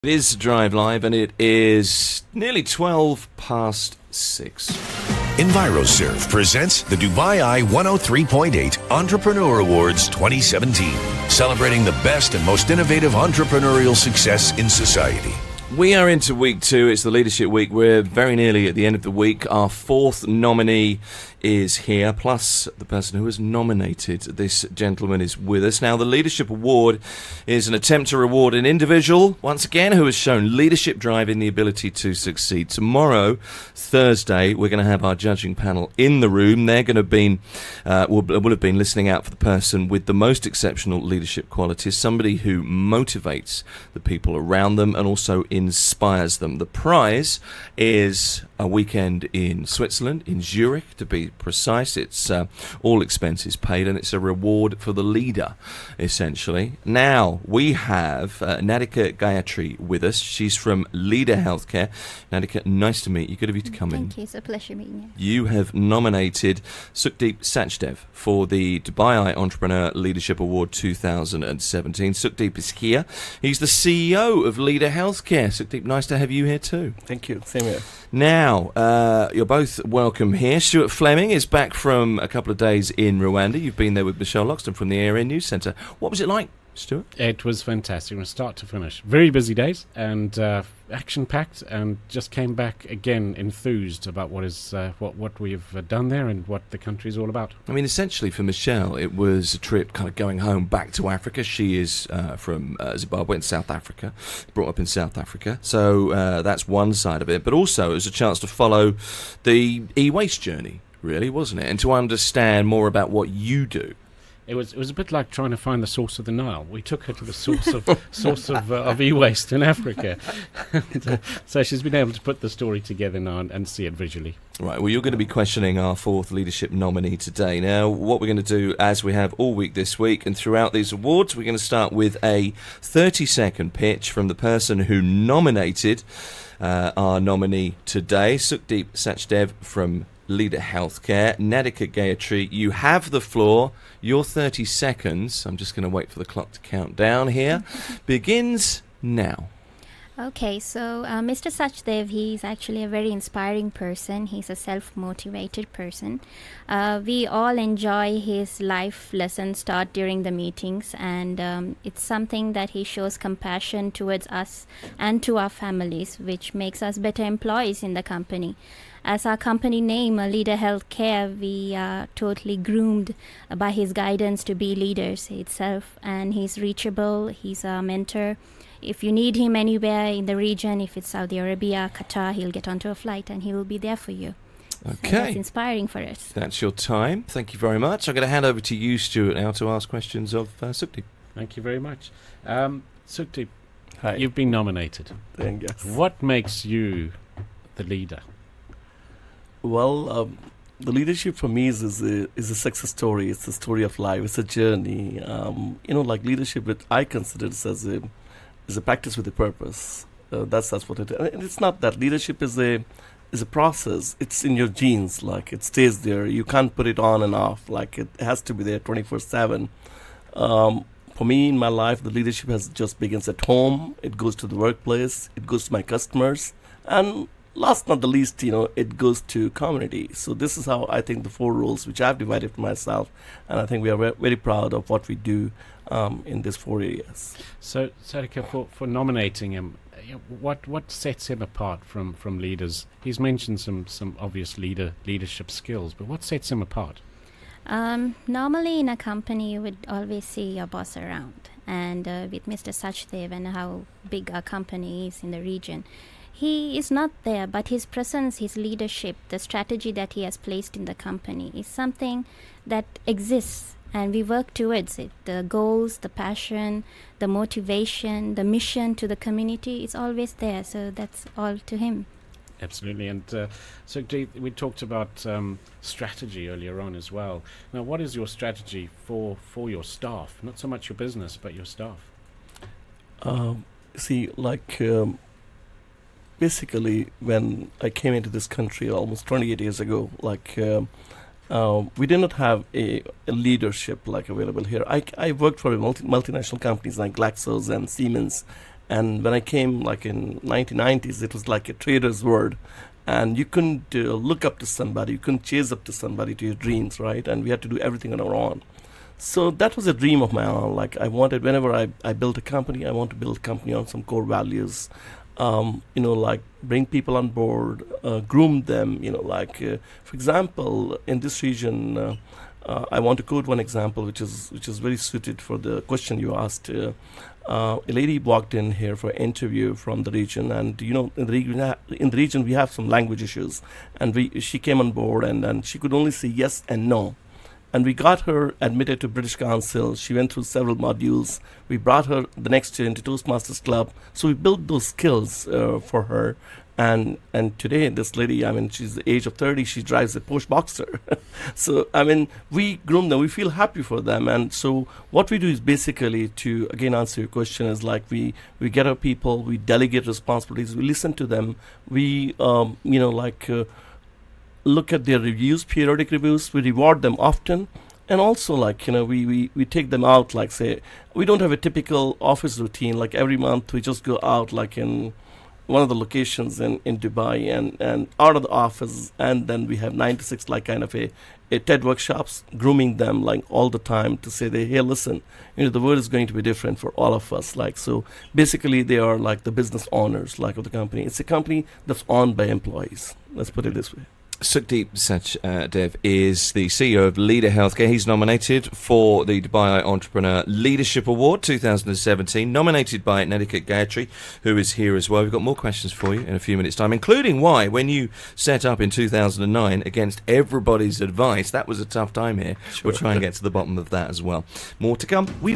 Dubai Eye 103.8 Entrepreneurship Awards - Leadership, Sukhdeep Sachdev
Sukhdeep Sachdev, CEO of Leader Healthcare Group, was nominated by his employee Nadeeka Gayathrie, who says his compassion and guidance are what sets him apart as a leader. Sachdev says his focus on building skills and grooming his employees is one of the keys to his success.